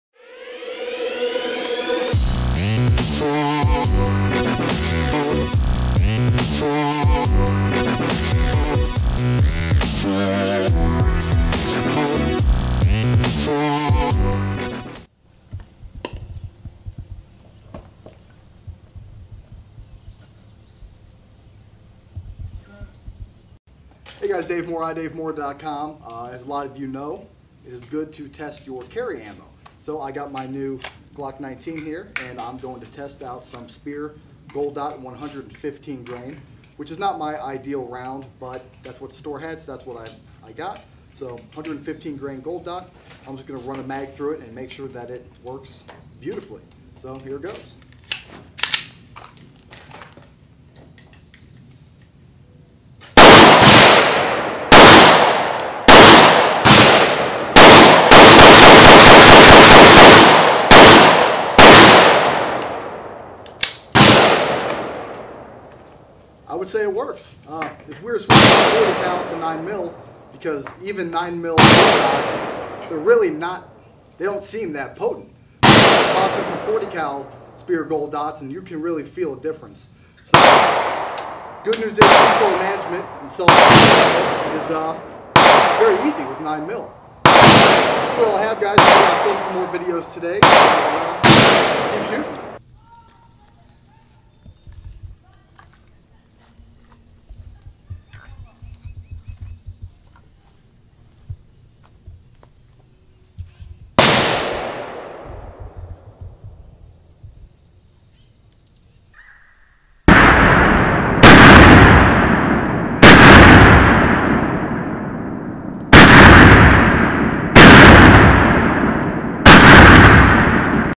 CCW Glock 19 and Gold Dot
You should always test your carry ammo, so in this video I run some of my Speer Gold Dot 115 grain 9mm rounds through my new Glock 19, and of course, it works flawlessly.

Or, click the link on the right side of my YouTube page!

for all your gear needs!